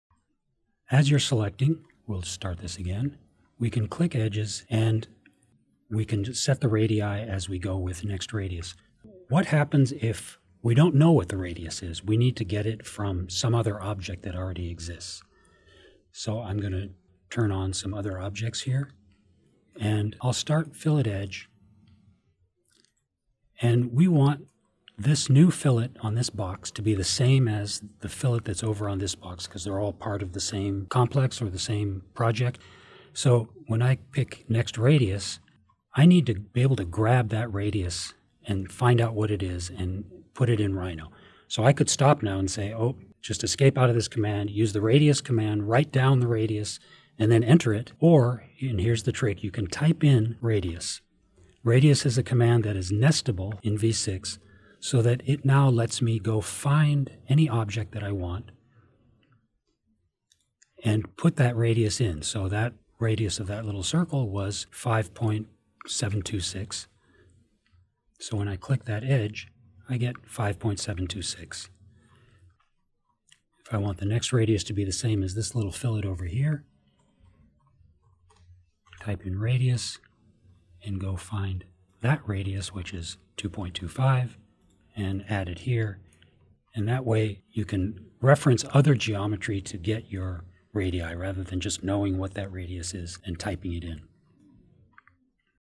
4 Advanced Fillet - Referencing a Radius
What happens when the radius you need to apply needs to match the radius on an adjacent object? Learn how to reference a radius without having to cancel out of the FilletEdge command.